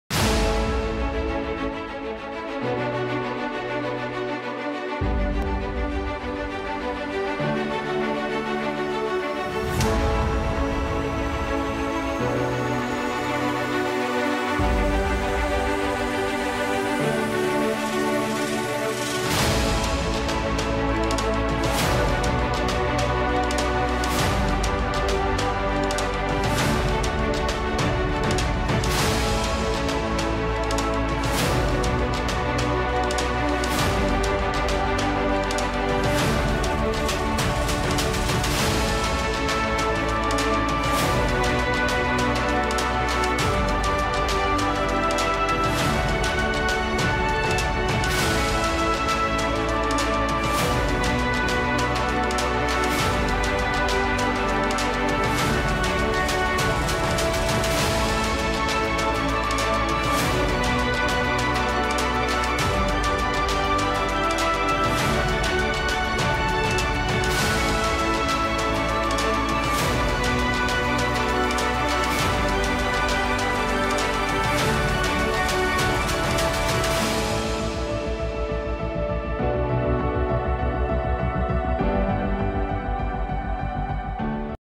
أهم محاور الجلسة الأولى باليوم الثاني من المؤتمر الاقتصادي
الجلسة الأولى باليوم الثاني من المؤتمر الاقتصادي.. تحت عنوان وثيقة سياسة ملكية الدولة ودعم سياسات المنافسة